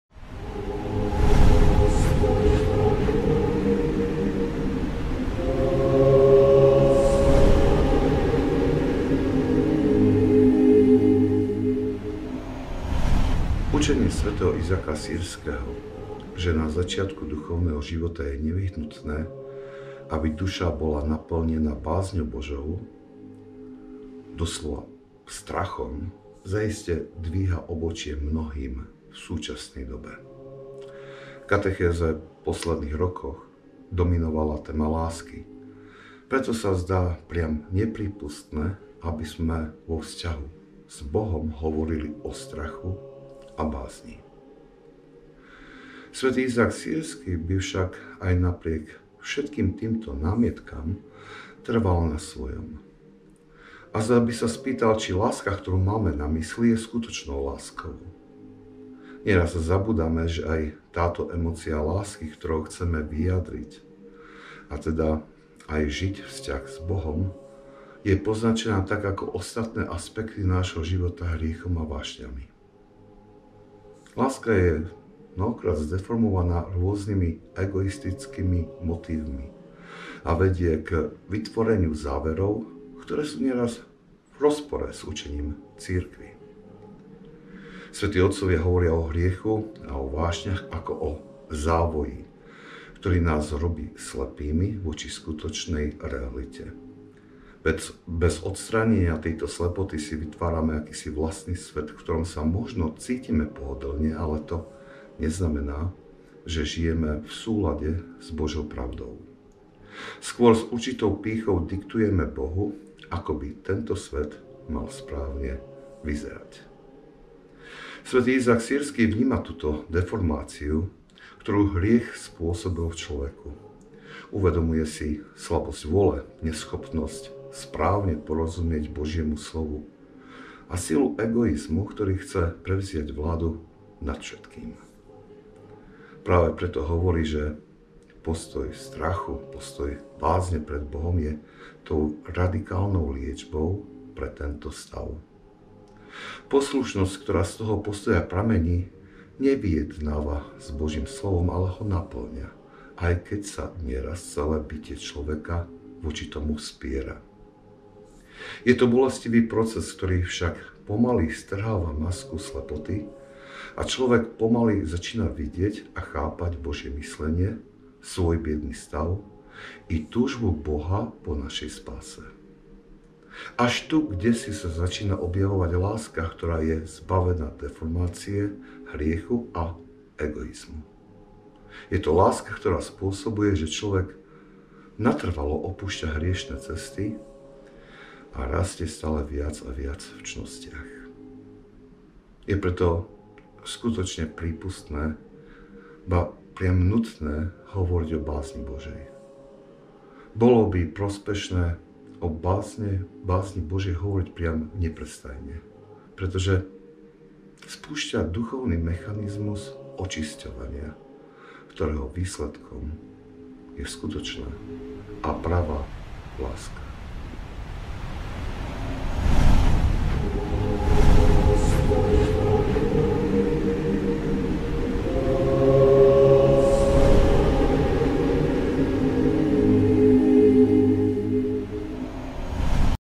Je nutné hovoriť o bázni Božej - OTCOVIA PÚŠTE - SV. IZÁK SÝRSKY (98)
Je prospešné o bázni Božej hovoriť priam neprestajne, pretože spúšťa duchovný mechanizmus očisťovania, ktorého výsledkom je skutočná a pravá láska.